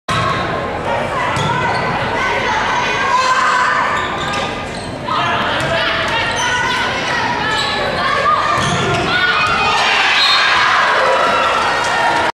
Freshman Setter Blocks Senior Outside Hitter....Again (Hubbard)
I blocked another senior outside hitter!